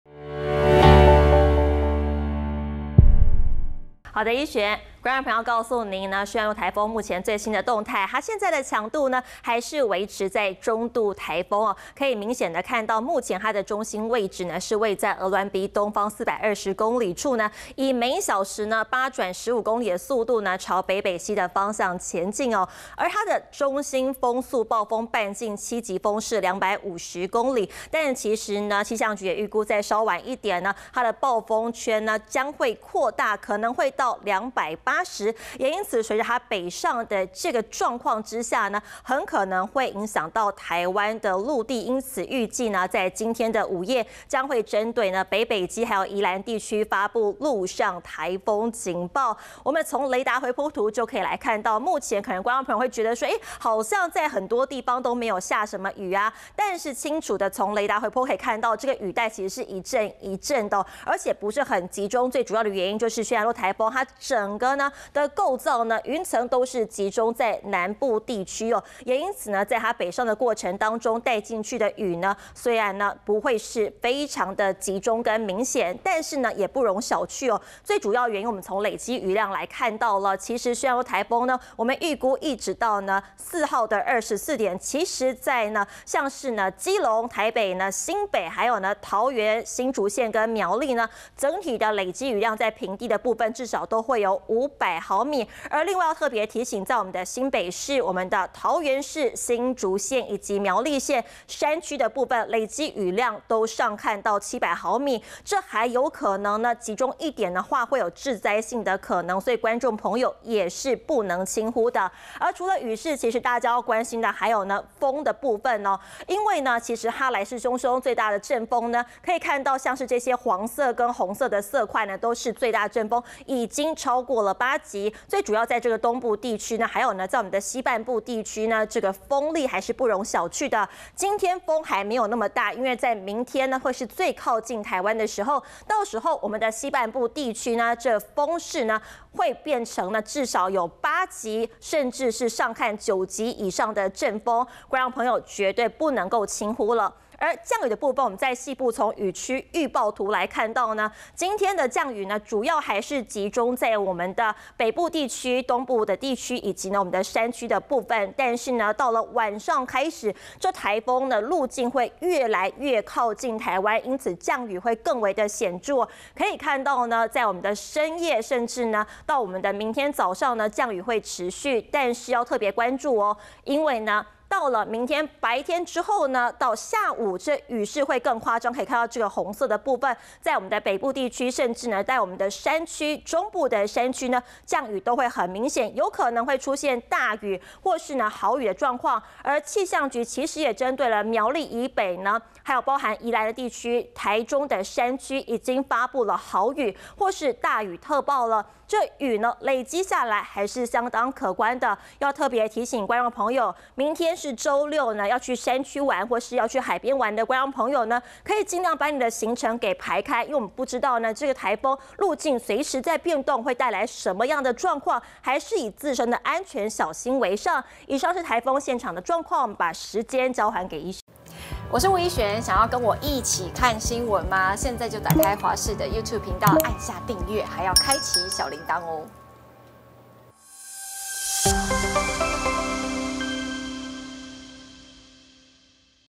軒嵐諾若路徑偏西 最快"今天午夜"發陸警｜華視新聞 20220902
關心中度颱風軒嵐諾，最快今午夜發布陸上警報，目前最新的颱風動態，交給主播宋燕旻。